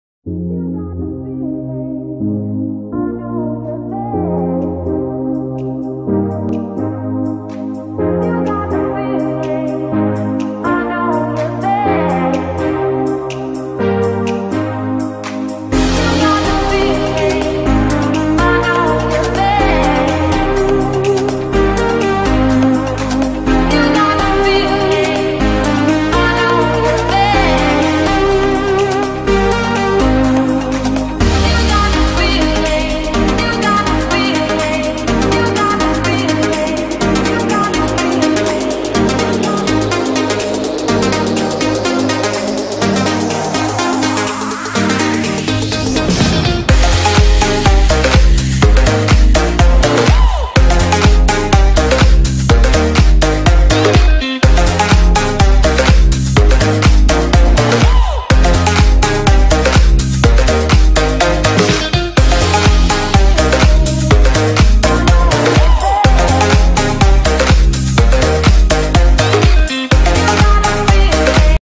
How inventor change life ||physical big items but smaller in our phones
This video well show  you how scientist and inventor work hard to make our life more efficient and comfortable.